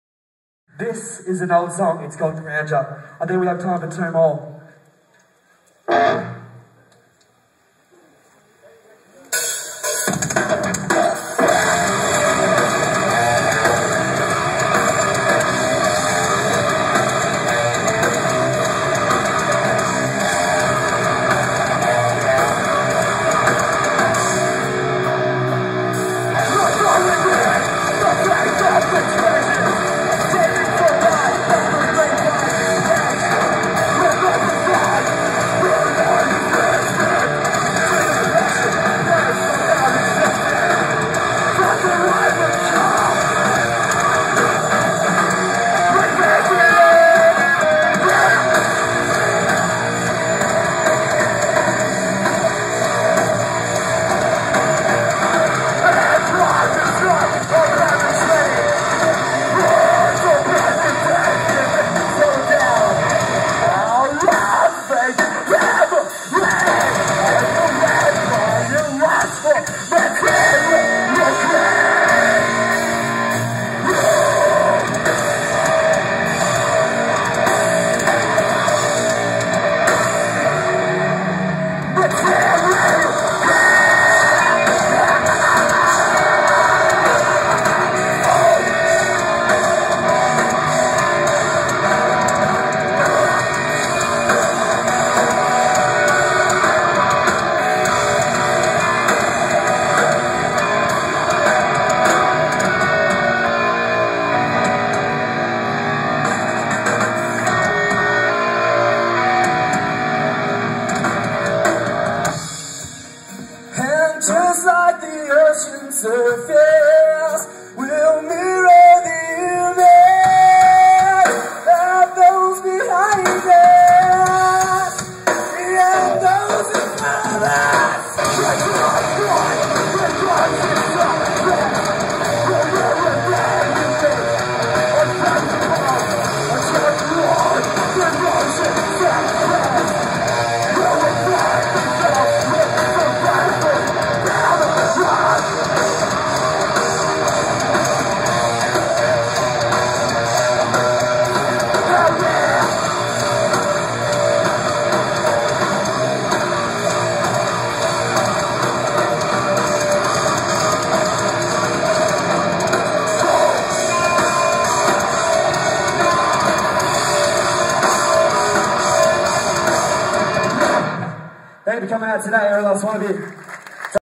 Ocean Grove - Live in Mount Gambier 17/2/13
Awesome Melbourne hardcore band Ocean Grove perform for their first time in South Australia at Mount Gambier's Old Jail in support of local hardcore band Sierra's EP launch. Thanks for making the trip fellas and performing outdoors with no shade in 35 degrees!!! F**kin' HOT